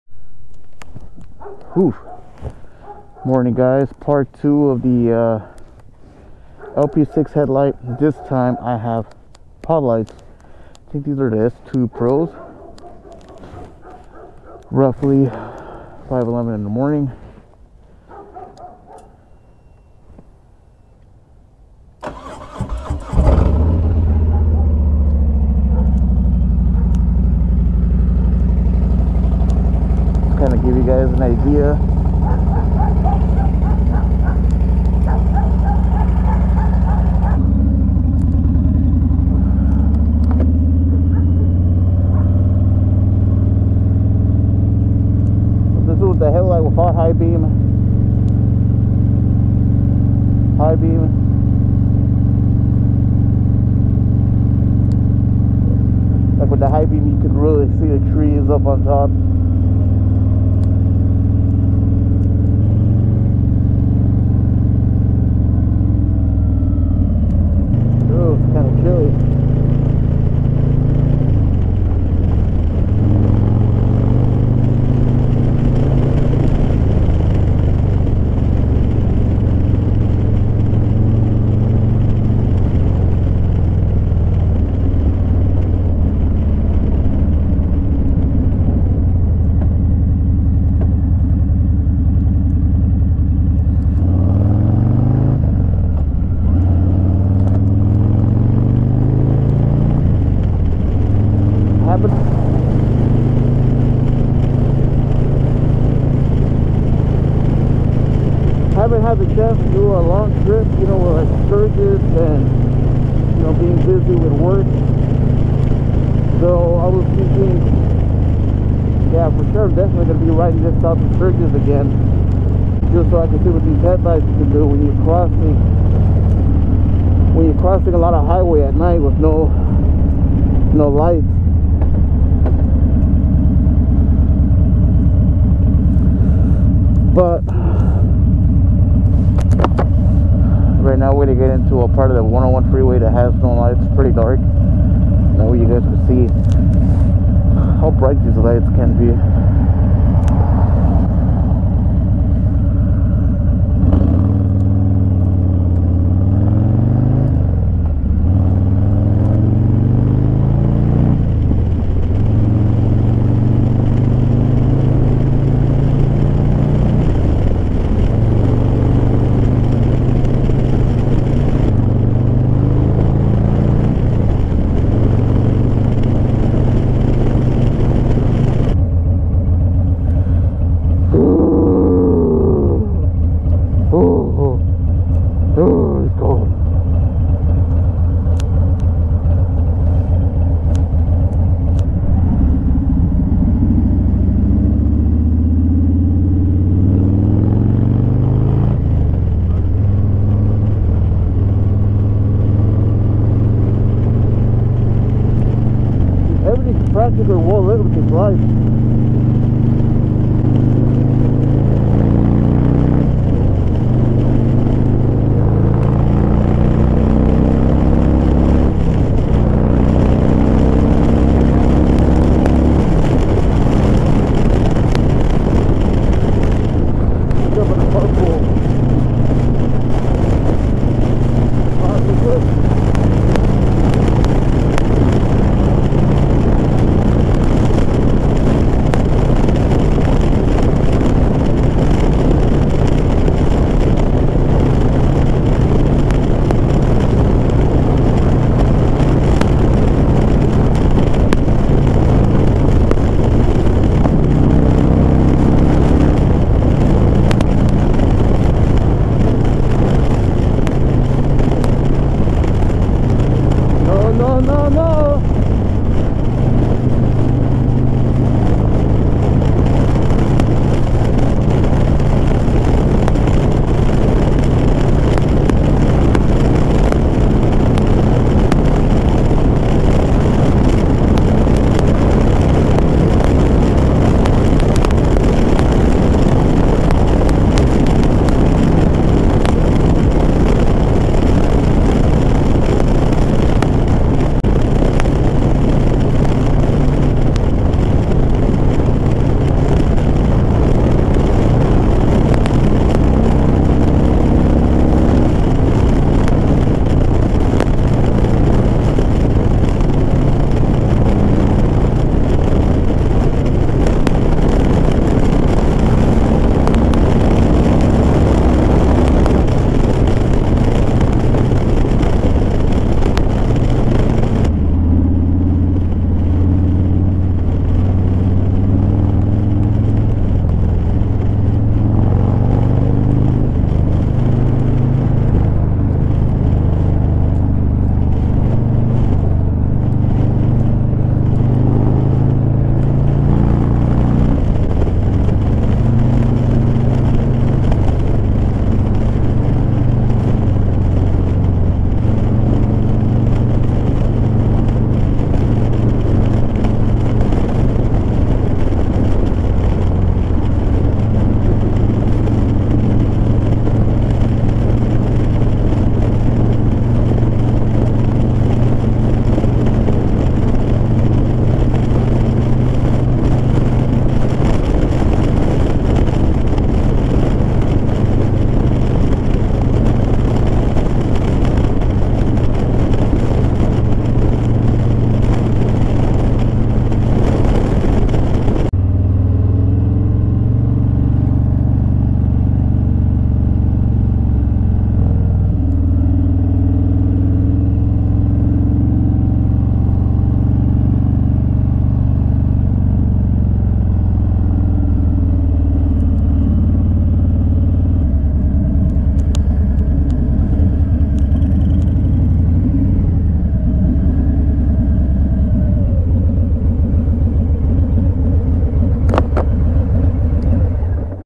Baja Designs LP6 S2pro Preview
Want to know just how bright the LP6 paired with S2 Pro Amber Pods can be? Hopefully this video can you with your decision, Yes they are Bright.